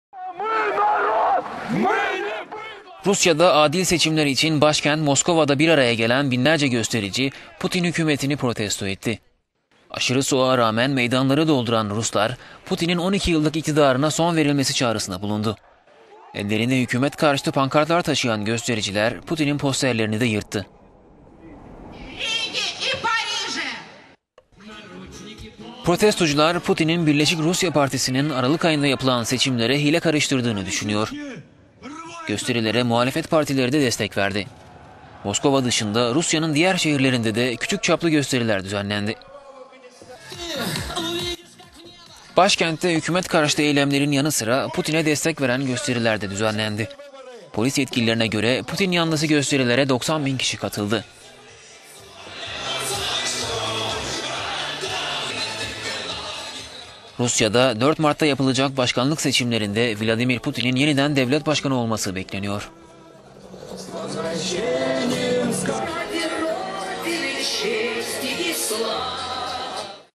Moskova'da Putin karşıtı ve yanlısı gösteriler
Rusya'da adil seçimleri için başkent Moskova'da toplanan binlerce gösterici, Putin hükümetini protesto etti. Aşırı soğuğa rağmen meydanları dolduran Ruslar, Putin'in 12 yıllık iktidarına son verilmesi çağrısında bulundu.

Ellerinde hükümet karşıtı pankartlar taşıyan göstericiler, Putin'in posterlerini de yırttı. Protestocular, Putin'in Birleşik Rusya Partisi'nin Aralık ayında yapılan seçimlere hile karıştırdığını düşünüyor. Gösterilere muhalefet partileri de destek verdi.